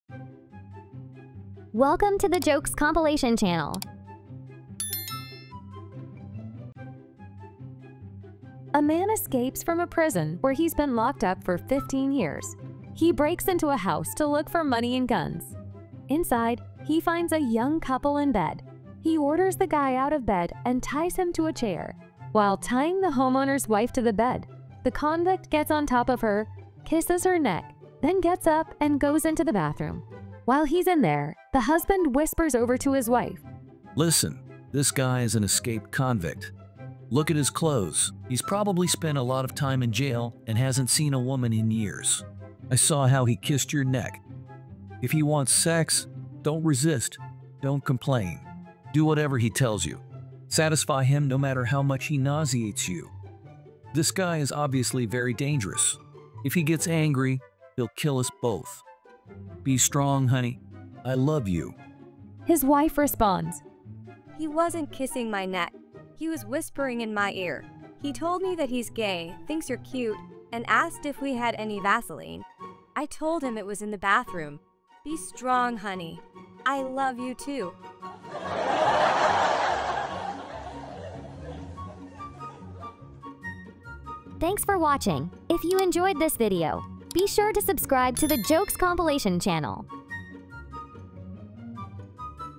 😁 Funny Jokes: A man escapes from a prison where he's been locked up for 15 years. He breaks...
😂 Try Not To Laugh: A man escapes from a prison where he's been locked up for 15 years. He breaks into a house to look for money and guns...

💻 Programs we use: Photoshop, Adobe Premiere Pro, canva.com, Audacity, Macbook Pro M3 Pro.

⚠️ All videos are self-produced by us. Copying without permission is prohibited!